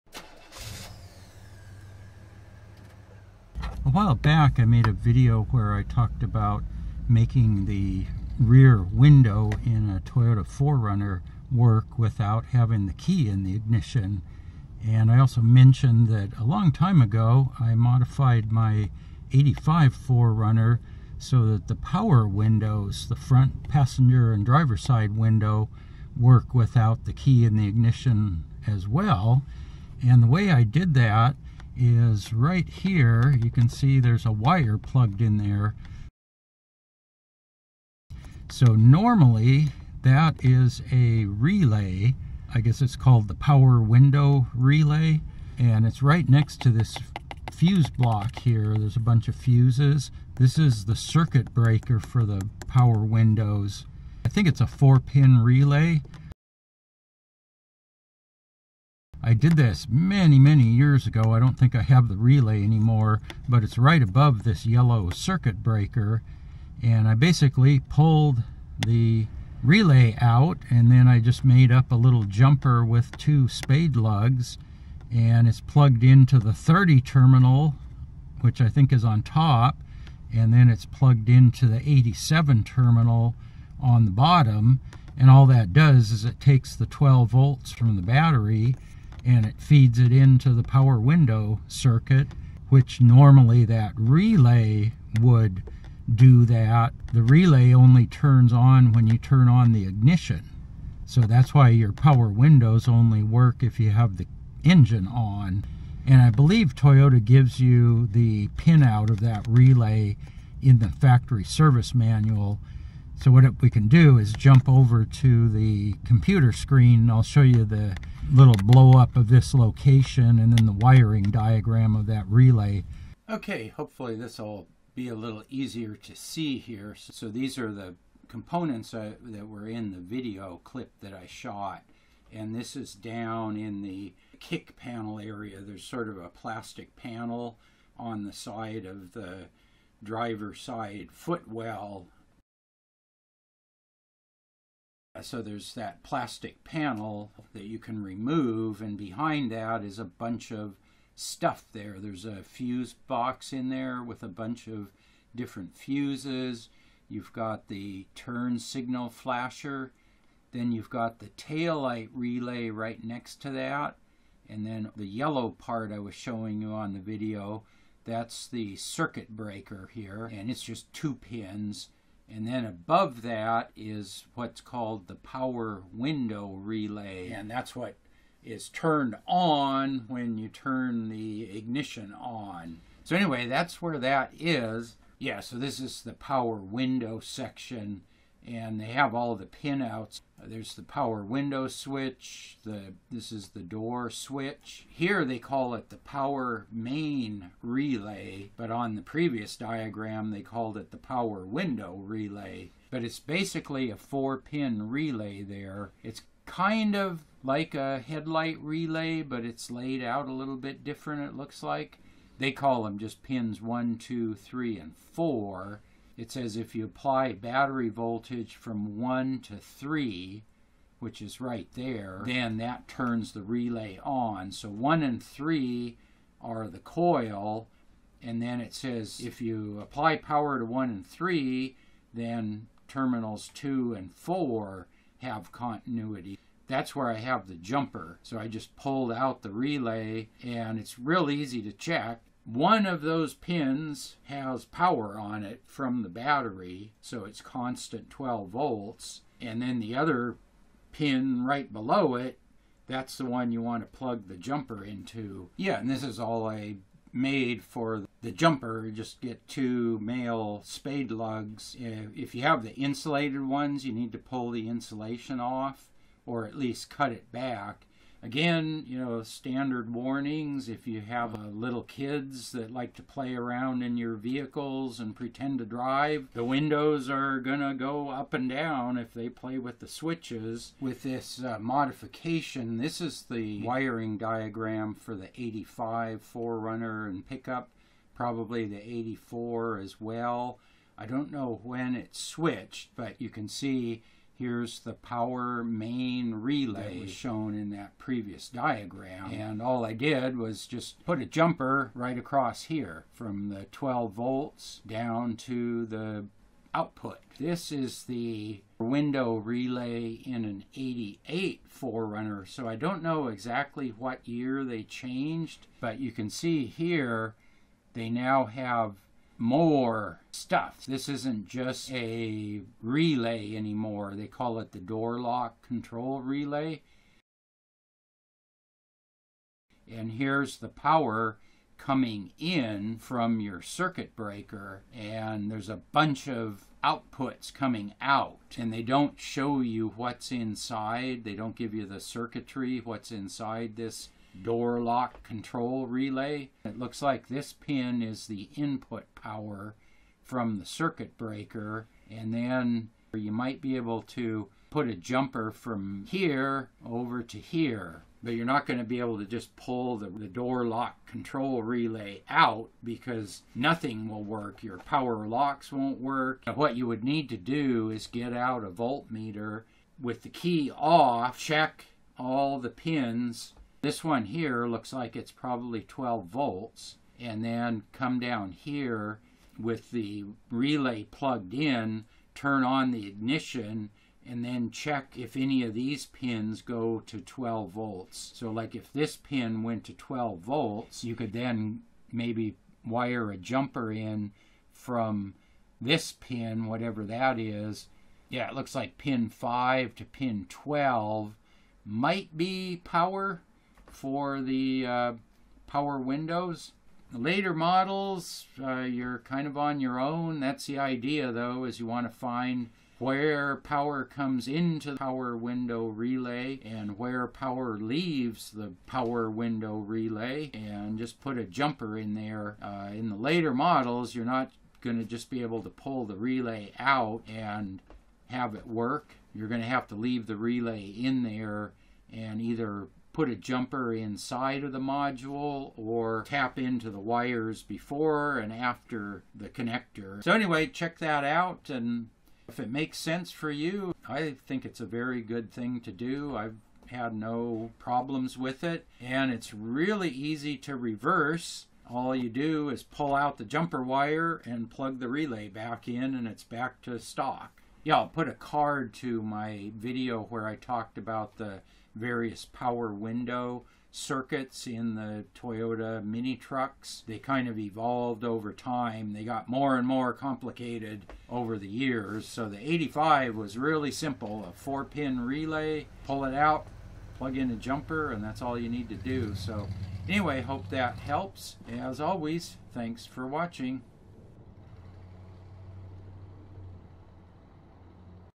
Toyota 4Runner / Pickup / Hilux /Surf Power Window Relay Mod
Modifying the power windows to work without the key in the ignition on  an 80's vintage Toyota 4Runner, Pckup, Hilux or Hilux Surf.

I did this modification some 25 years ago after studying the Factory Service Manual wiring diagram.  I simply pulled the original Power Main Relay and placed a wire jumper between pins 2 & 4.  This sends 12V to the power window circuit directly.  The factory has that relay coil wired to ignition power, so the power windows won't operate unless the engine is running.

For my needs, having power windows work all the time is vary handy.  If I forget to roll them up, just flip the switch and up they go; no searching for the keys.  If you try this mod and don't like it, just pull the jumper and plug the relay back in and you have the factory setup again.

Now, if you have small children who like to play in your truck, this may not be a wise thing to do.  The '85 and earlier models had no power window lock feature, like the later models added.  Also, this may reduce vehicle security.